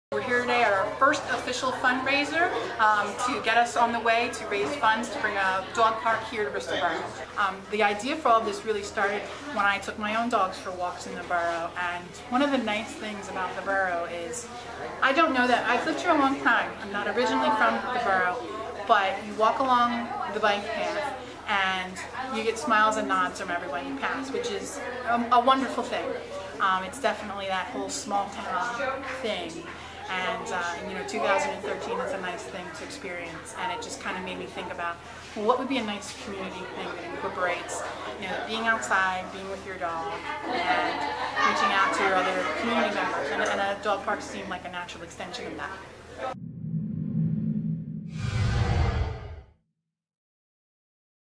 Bristol Bark Park
Christa Barlow, president of Bristol Borough Bark Park, a campagin to raise money for a dog park in Bristol Borough, speaks about a fundraiser held Sunday afternoon at Grundy Recreation Center Ice Skating Rink for the park.